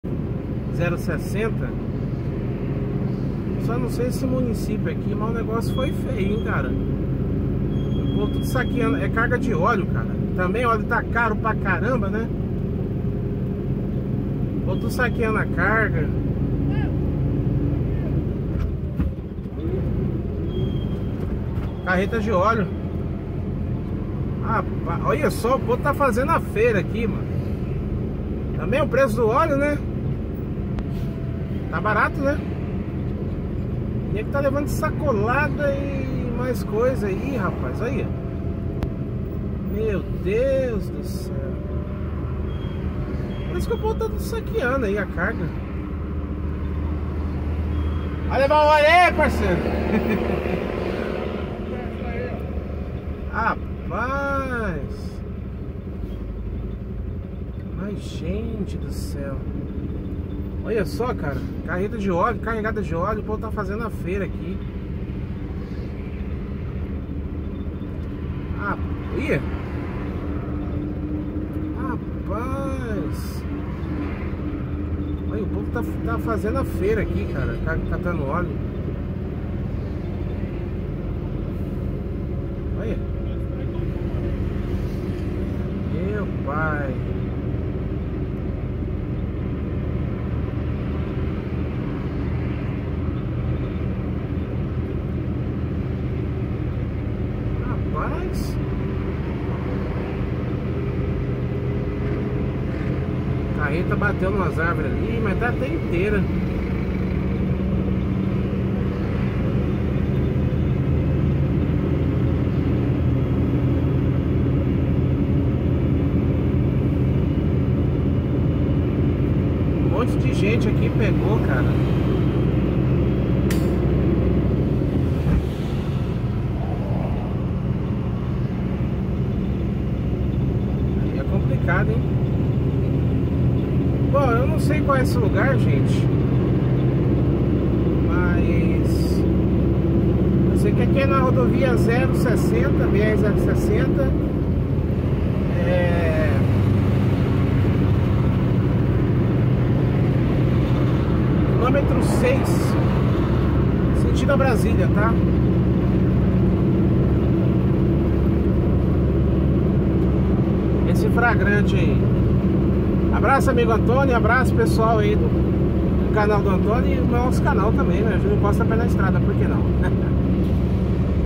Chegando em Brasília BR060 Roubo Carreta carregada de óleo de soja !
Vídeo de PATRÍCIA